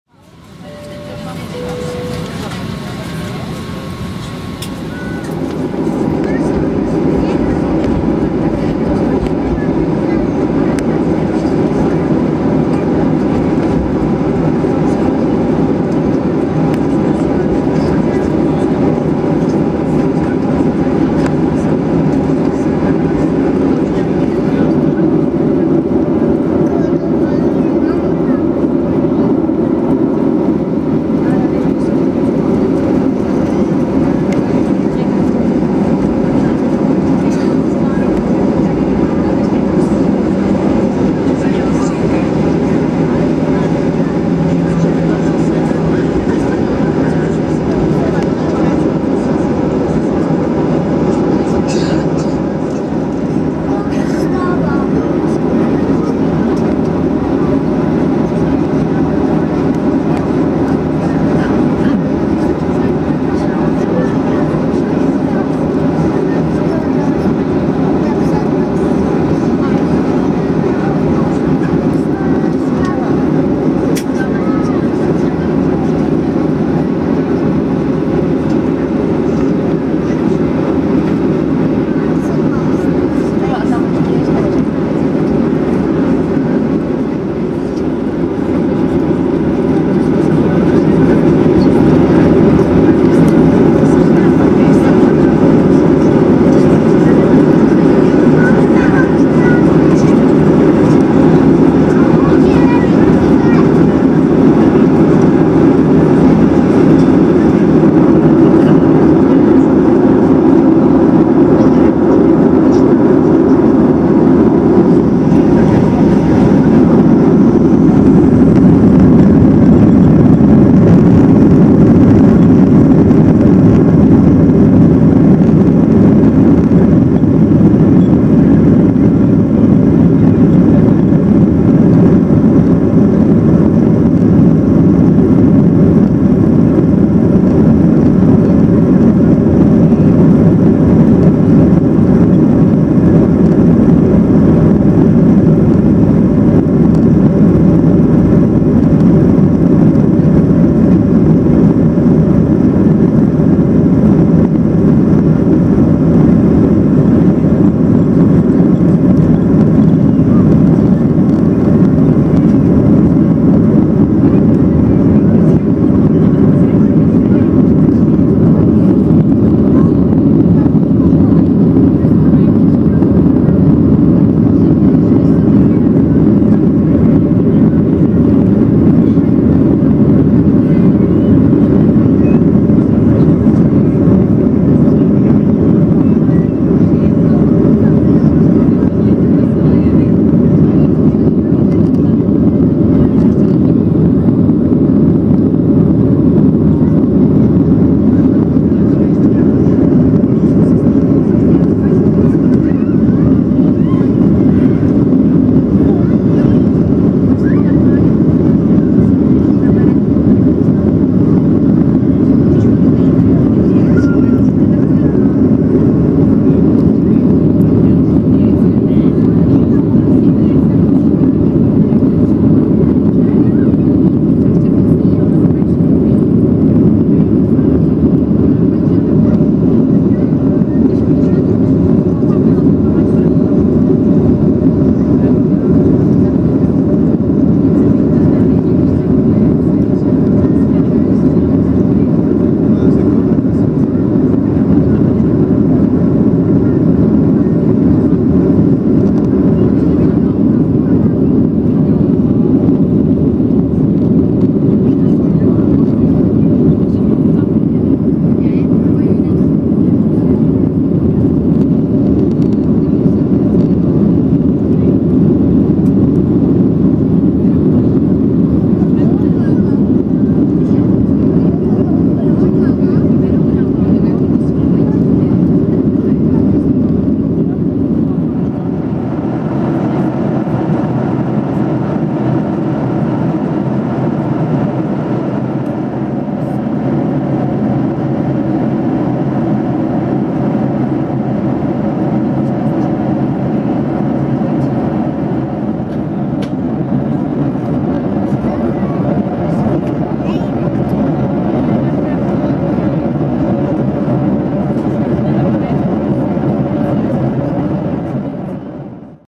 Travel Service take-off from Prague Airport
Travel Service from Prague Airport PRG to Rhodes RHO Boeing 737-86N Reg: OK-TVK
take-off, flight, cloudiness 4:48 storm 5:00...cabin video
Letiště Praha LKPR date: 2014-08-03